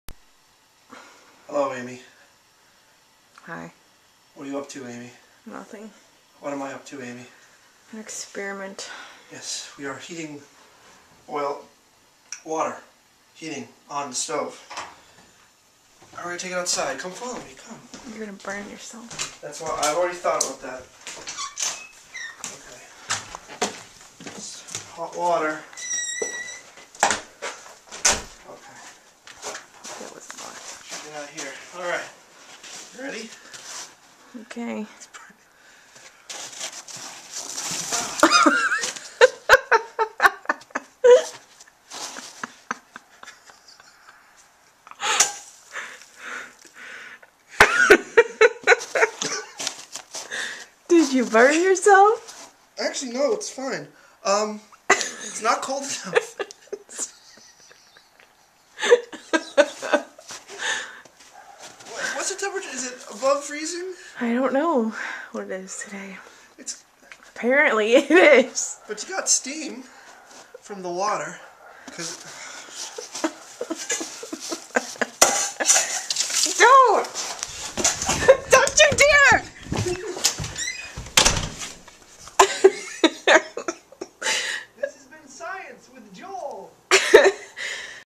237 - Science with Joel (FAIL)
So I've seen vids on you tube where people throw hot water into the cold air and its turns into snow and falls to the ground. A week or two ago before this video I heated up some water, threw it up into the sky, and it turned into snow. ... HOWEVER, as you can see in this video ... ummm ... yeah. lol